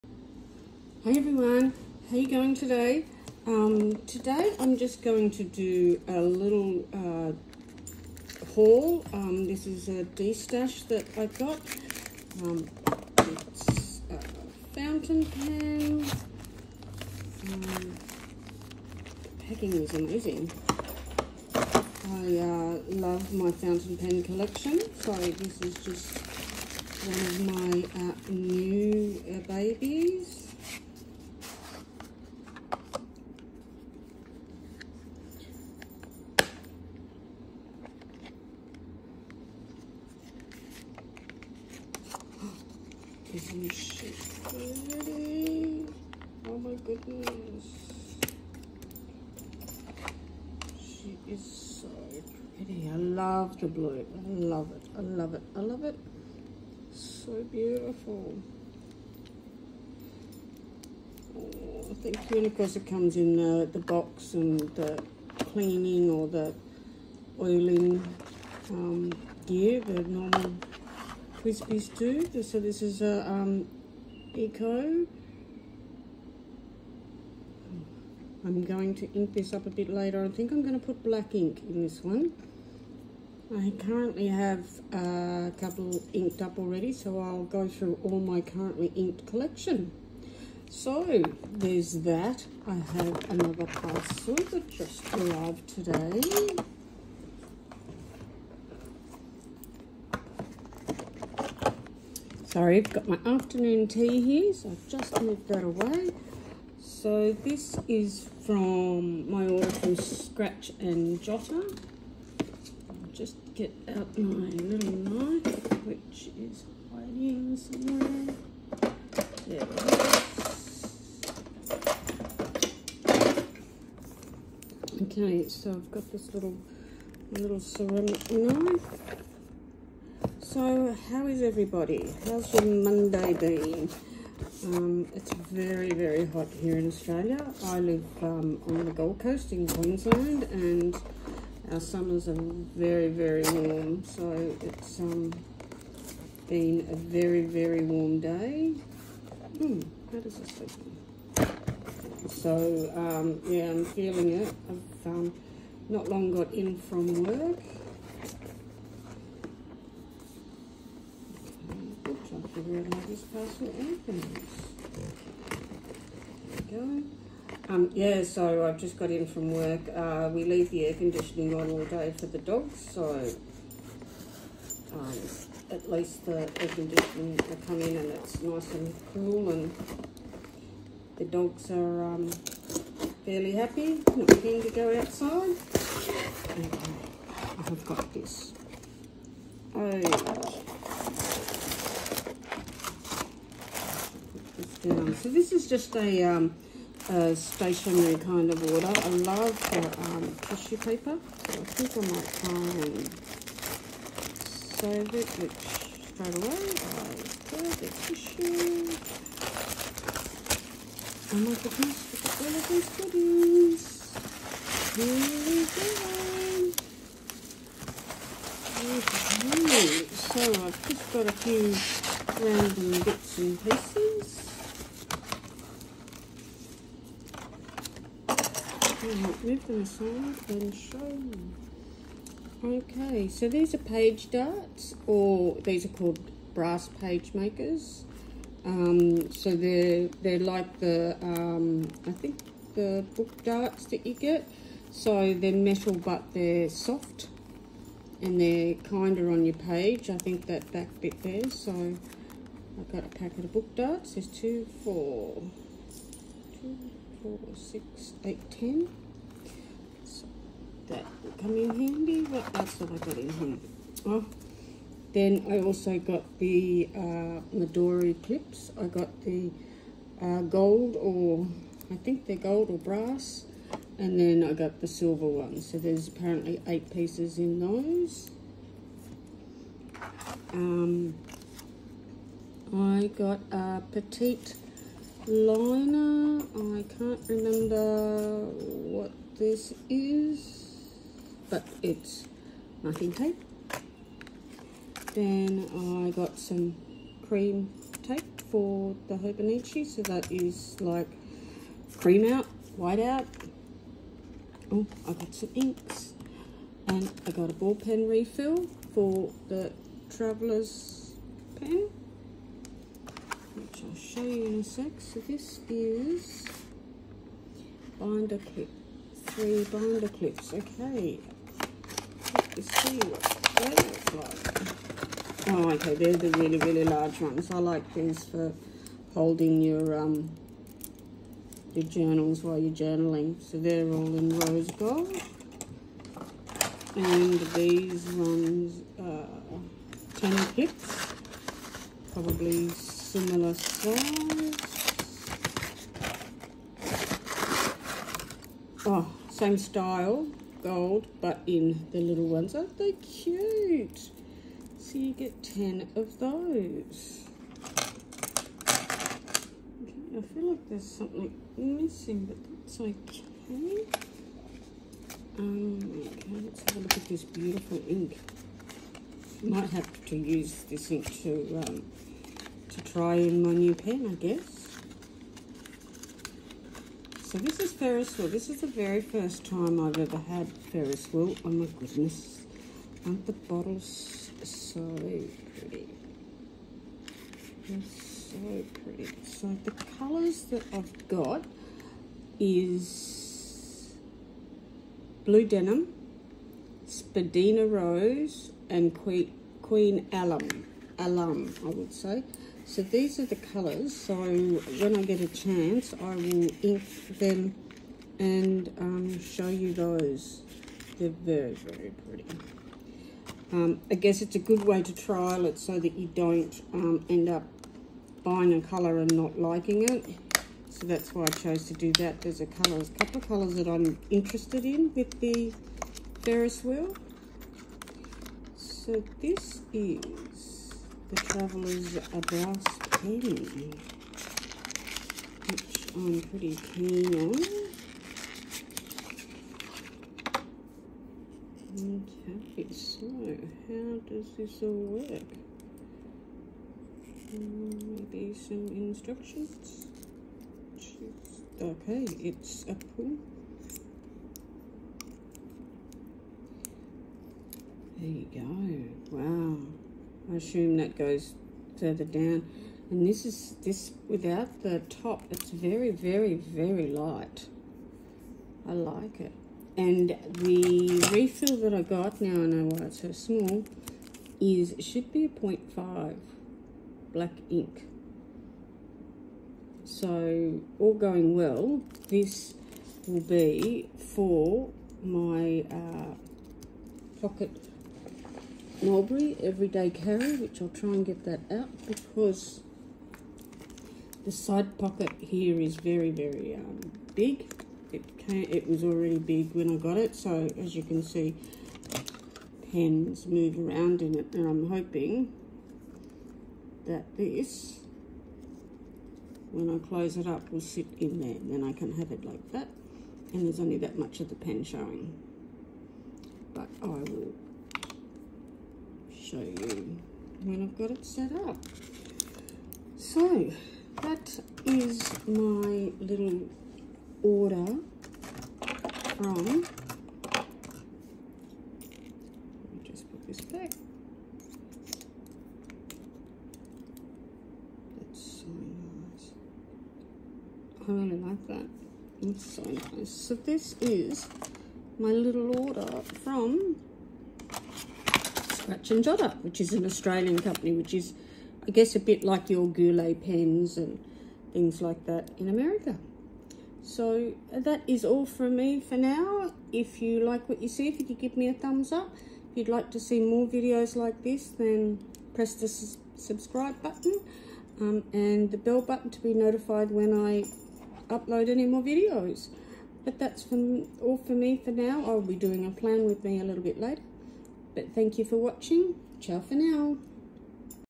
Very Quick haul. Twsbi Eco, Ferris Wheel Ink Brass pen and clips from Scratch & Jotter
Just wanted to share my quick haul of stationary and pen and inks

Scratch & Jotter is one of many suppliers for these products the service is so quick I ordered these on the 5th of Jan and picked up on the 9th take out two days for weekend and that is two day turnaround 🙌🏼🦋